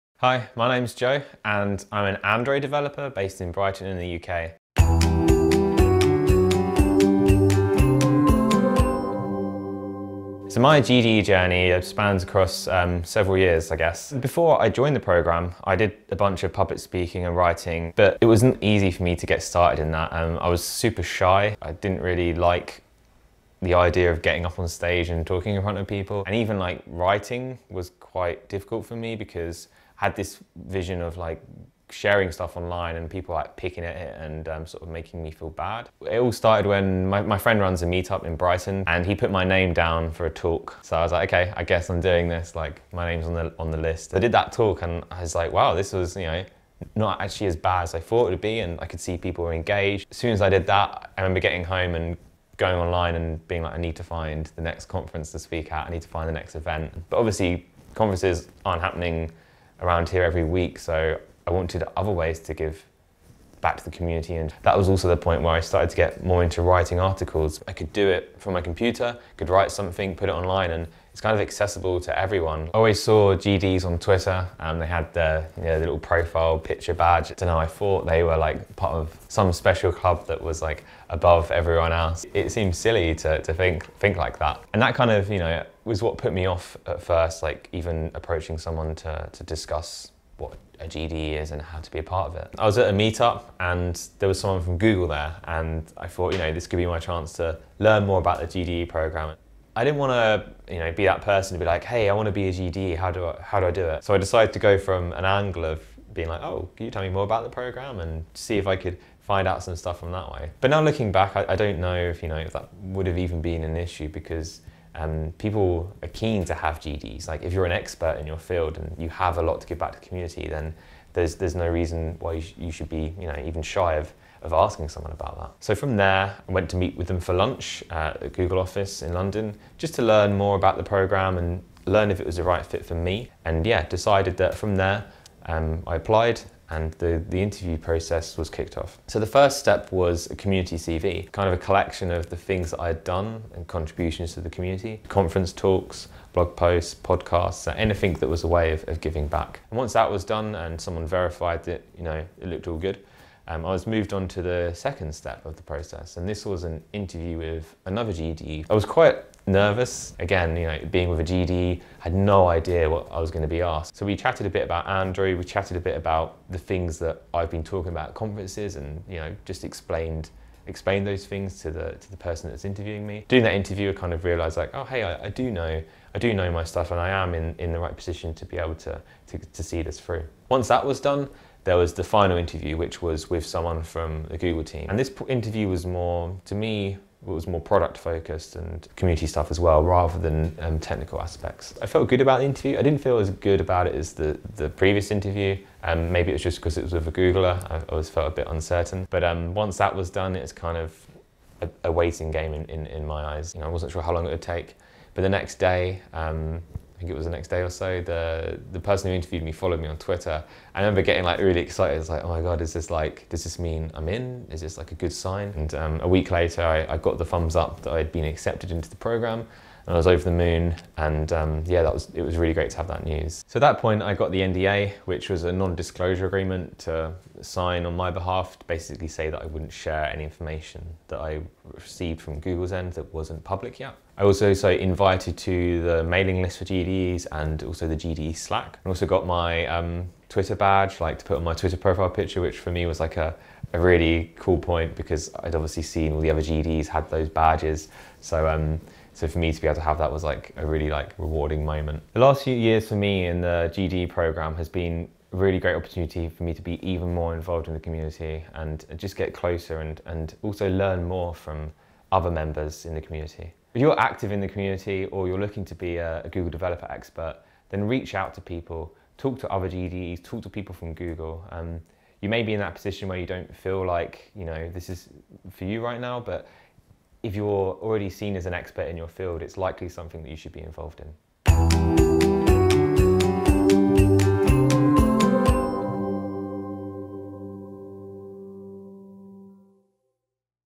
My GDE journey - Joe Birch
Watch now and hear Joe Birch discuss his journey to becoming a Google Developers Expert!